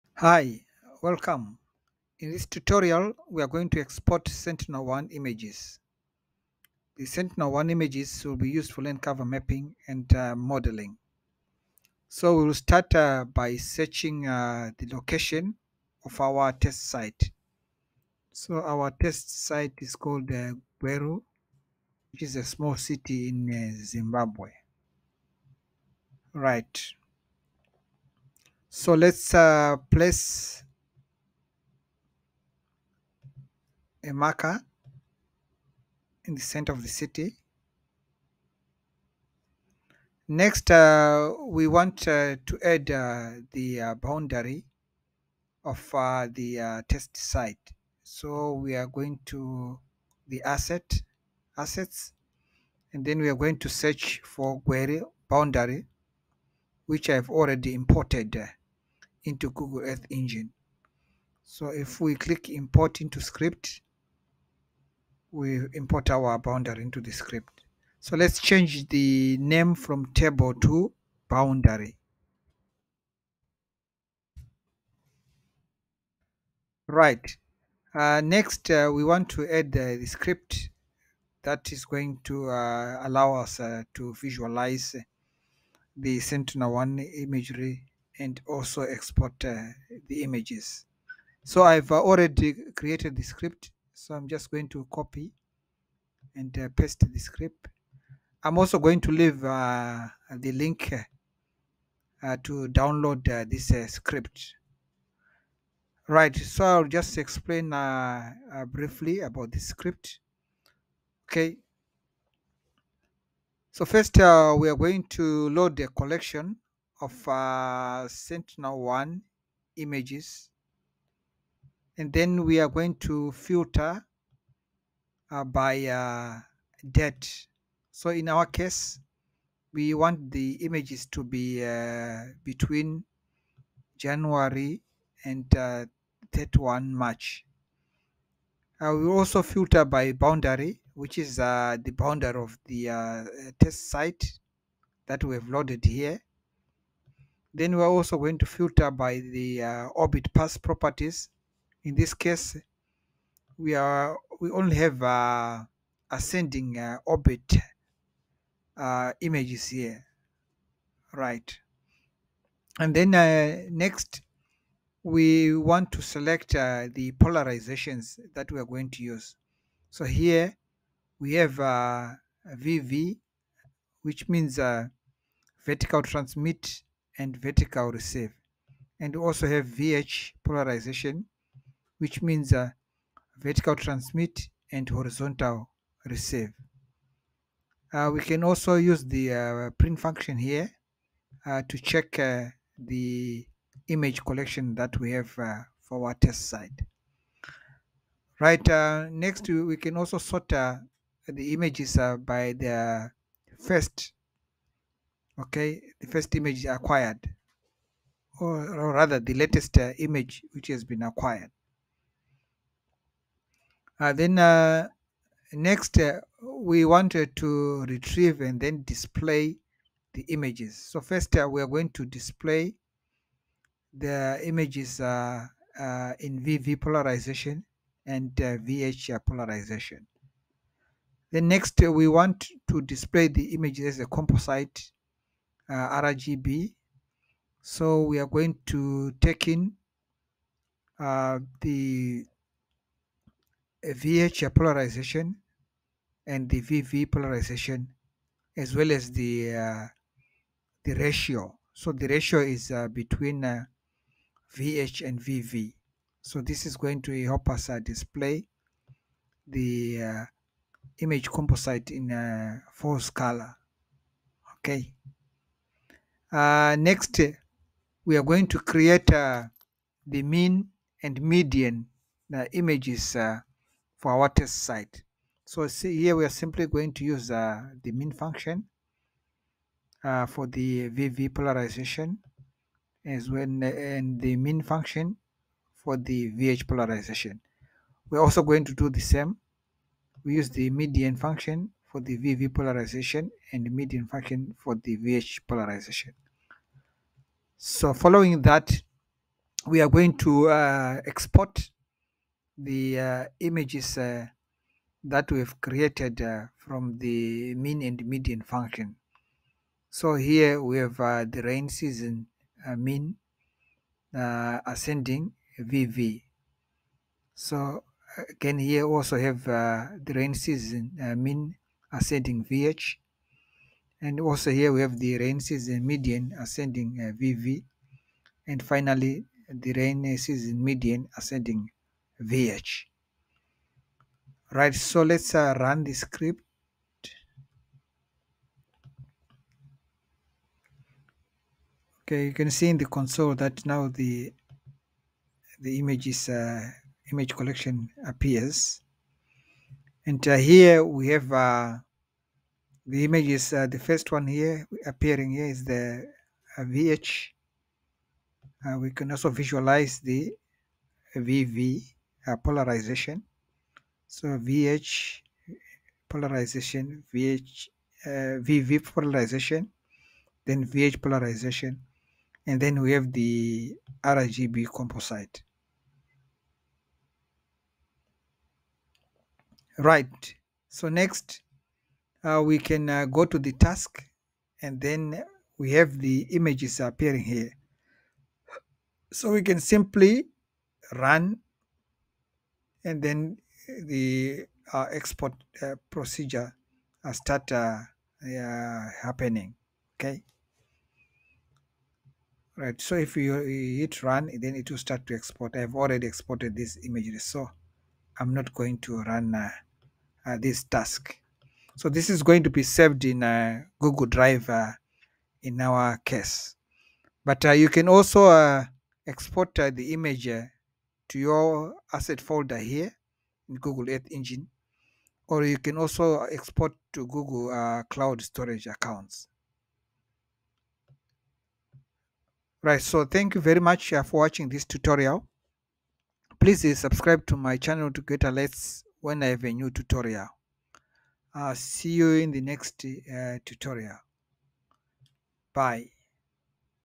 Lab 2 Exporting Sentinel 1 Imagery from GEE
This tutorial shows how to export Sentinel-1 data from GEE.